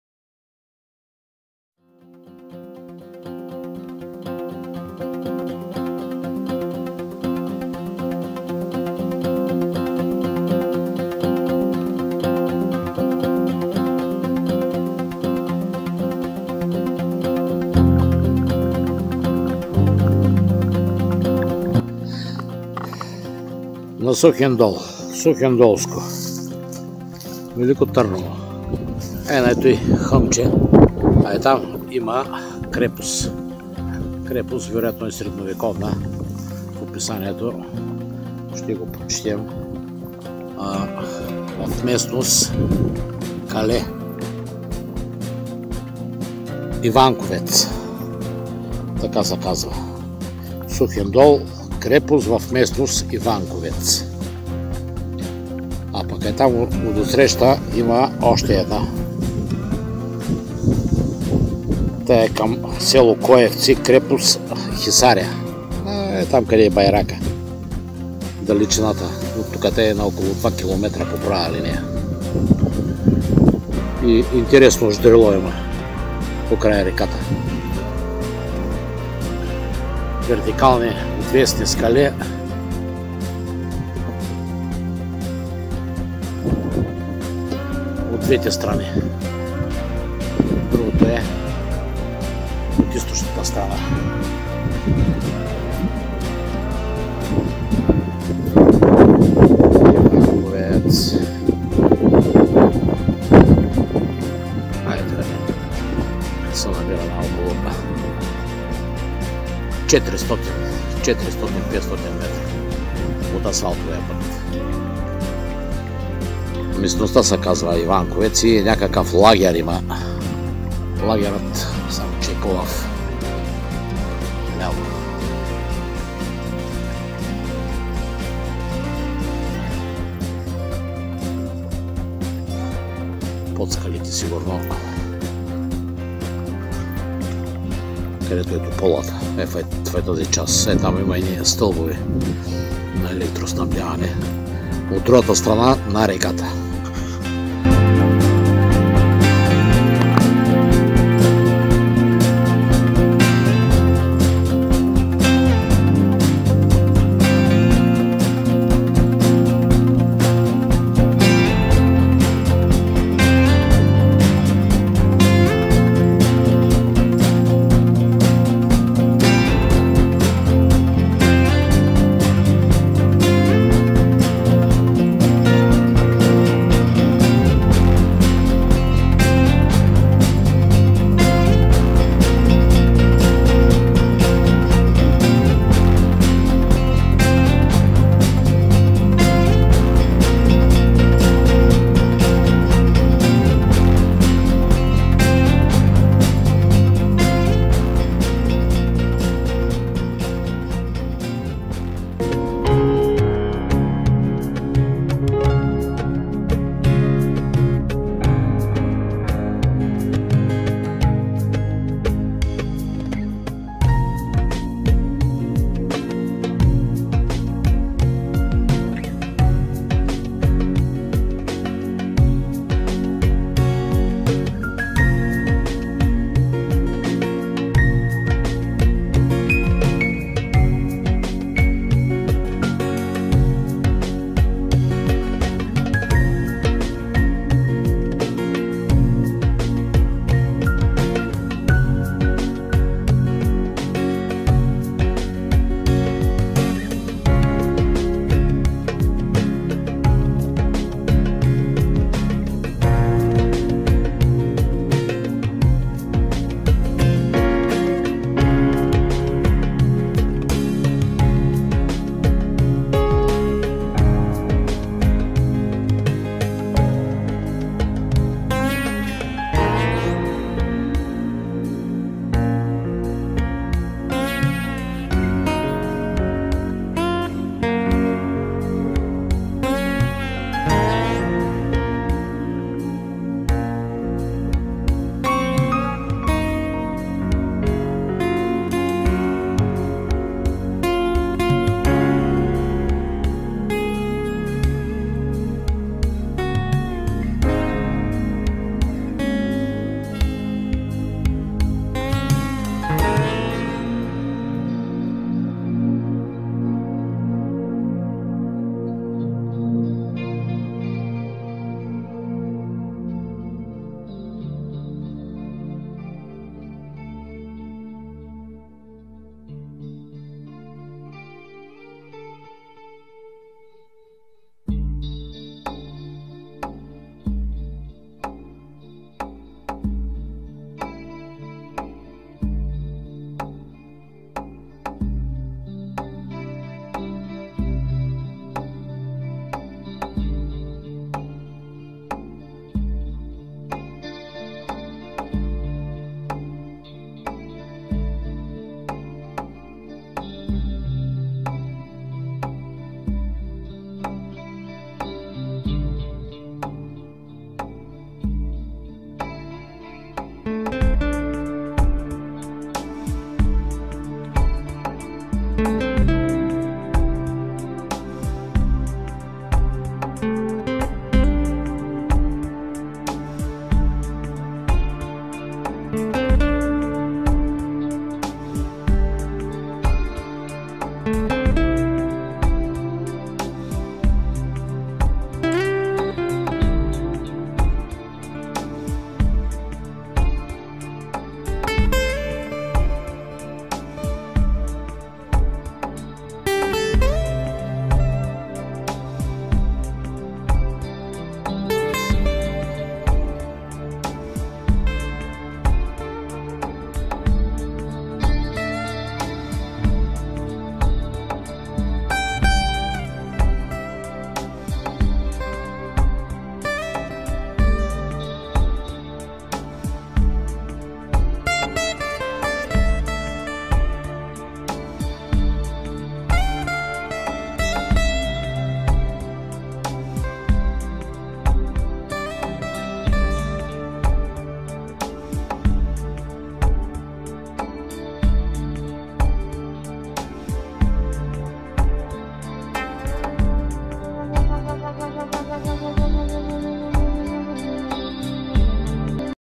Сухиндол – Тракийска и римска крепост в м. „Иванковец” 2022
Тракийска и римска крепост Иванковец се намира в едноименната местност, на 4.12 km южно по права линия от центъра на град Сухиндол. Издигната е в североизточния край на дълъг рид, с много стръмни склонове от север и отвесни от изток и юг. Достъпна е единствено от югозапад по билото на рида, където през широка седловина се стига до укреплението. От изток и югоизток ридът е ограничен от река Росица, а от север и северозапад- от ляв неин приток.

Твърдината Иванковец има издължена и тясна, триъгълна форма. Приблизителните и размери са 160х127х50 m. Крепостната стена е ориентирана в посока север- юг и се проследява под формата на насип с ширина 10 m и височина до 3-4 m. Градена е от местен, ломен камък, като спойка не се забелязва. Обща и дължина е около 50 m. Тя прегражда напълно достъпа до носа. В северната част на стената се забелязва седловина. Най- вероятно там е бил входът на Иванковец. Пред целия фронт на стената има следи от ров със запазена ширина от 5 m  и дълбочина от 1 m.

Вътрешността на крепостта е обрасла с ниски храсти, дървета и трева, което не позволява намирането на фрагменти от керамика и по- точното определяне на параметрите и. Вероятно обектът е тракийски, като е продължил да съществува и след римската инвазия.

Местоположение
Надморска височина: 204 m GPS координати: 43°08’55” С.Ш. и 25°10’32” И.Д.

Литература
Ковачев, Н. Местните названия от Севлиевско. София, 1961.